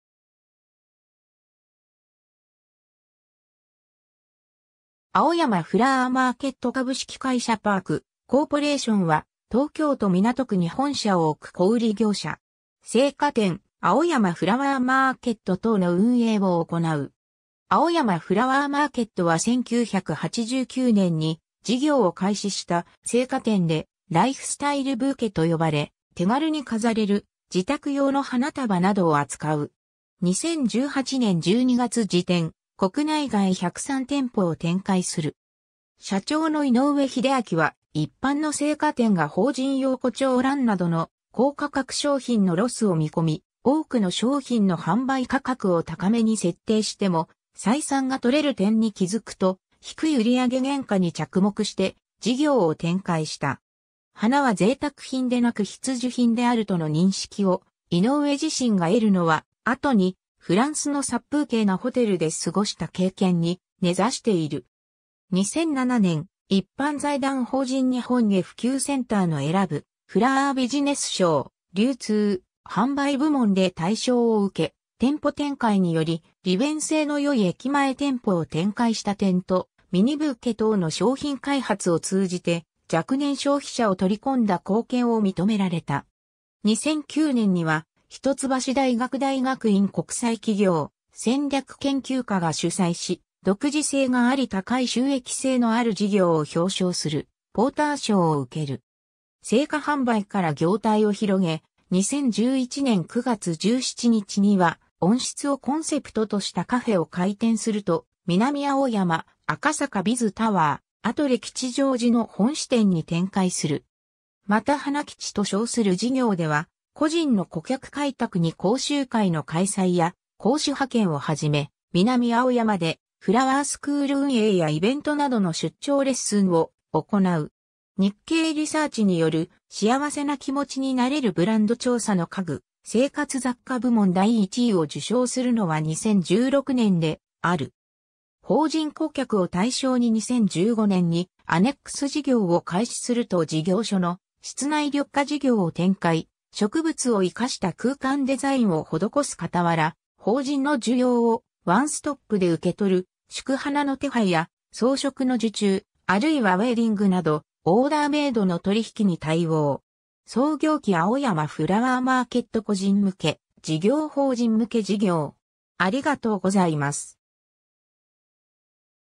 パーク・コーポレーション
花きの販売・流通に関わる組織・企業
青山フラワーマーケット（ジョイナス1F店） 株式会社パーク・コーポレーション（英: Park Corporation）は、東京都港区に本社を置く小売業者。
生花店「青山フラワーマーケット」等の運営を行う。
 青山フラワーマーケットは1989年に事業を開始した生花店で「ライフスタイルブーケ」と呼ばれ、手軽に飾れる自宅用の花束などを扱う。
2018年12月時点、国内外103店舗を展開する。
 社長の井上英明は一般の生花店が法人用胡蝶蘭などの高価格商品のロスを見込み、多くの商品の販売価格を高めに設定しても採算が取れる点に気づくと、低い売上原価に着目して事業を展開した。
花は贅沢品でなく必需品であるとの認識を井上自身が得るのは、後にフランスの殺風景なホテルで過ごした経験に根差している。
2007年、一般財団法人日本花普及センターの選ぶフラワービジネス賞「流通・販売部門」で大賞を受け、店舗展開により利便性のよい駅前店舗を展開した点と、ミニブーケ等の商品開発を通じて若年消費者を取り込んだ貢献を認められた。
 2009年には一橋大学大学院国際企業戦略研究科が主催し、独自性があり高い収益性のある事業を表彰するポーター賞を受ける。
生花販売から業態を広げ、2011年9月17日には温室をコンセプトとしたカフェ を開店すると、南青山、赤坂Bizタワー、アトレ吉祥寺の本支店に展開する。
またhana-kichi と称する事業では、個人の顧客開拓に講習会の開催や講師派遣を始め、南青山でフラワースクール運営やイベントなどの出張レッスンを行う。
日経リサーチによる「幸せな気持ちになれるブランド」調査の家具・生活雑貨部門第1位を受賞するのは2016年である。
 法人顧客を対象に2015年にANNEX事業を開始すると事業所の室内緑化事業を展開、植物を活かした空間デザインをほどこす かたわら、法人の需要をワンストップで受け取る祝花の手配や装飾の受注、あるいはウェディングなどオーダーメイドの取引に対応。
 創業期 青山フラワーマーケット 個人向け事業 法人向け事業